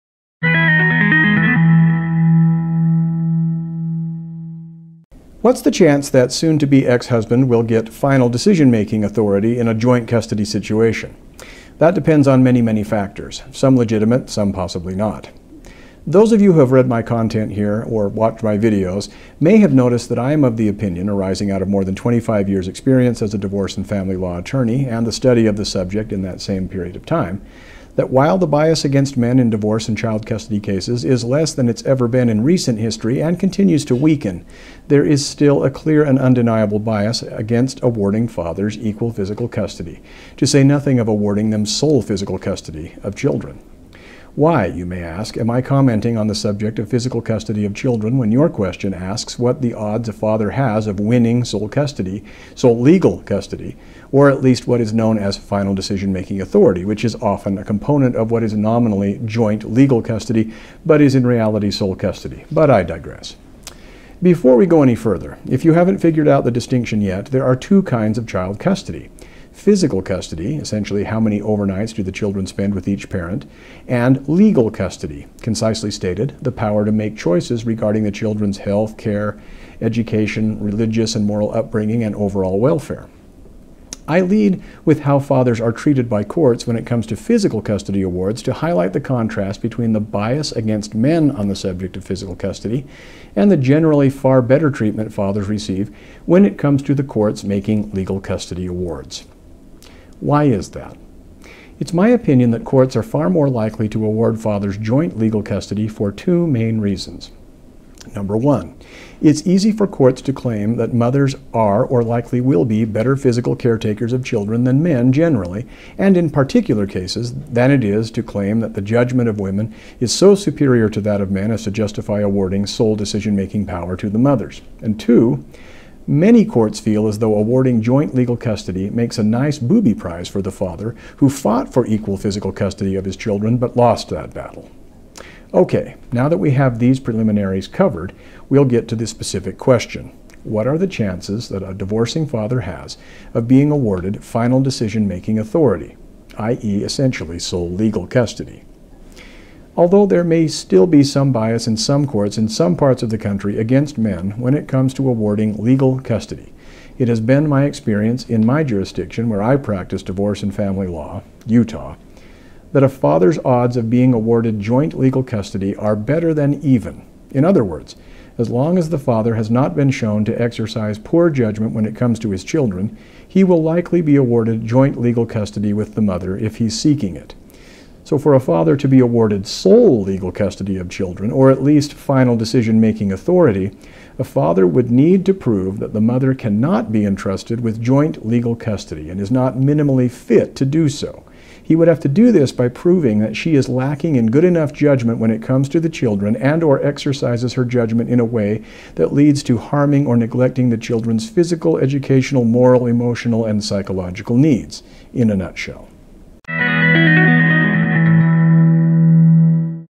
What’s the Chance That Soon-to Be Ex-husband Will Get “Final Decision Making” in Joint Custody?
That depends on many, many factors. Some legitimate, some possibly not.